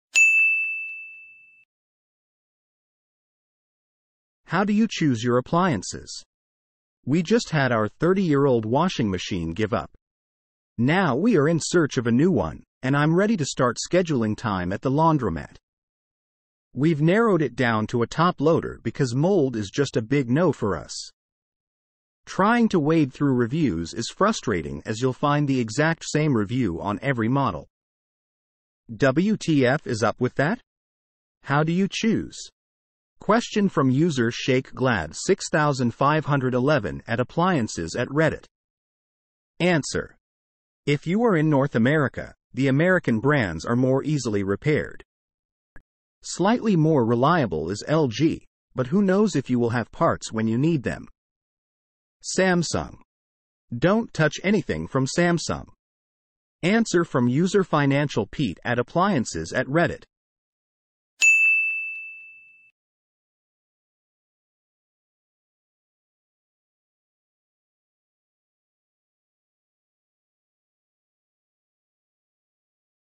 How do you choose your appliances?
We just had our 30-year-old washing machine give up. Now we are in search of a new one, and I'm ready to start scheduling time at the laundromat. We've narrowed it down to a top loader because mold is just a big NO for us.

Trying to wade through reviews is frustrating as you'll find the exact same review on every model.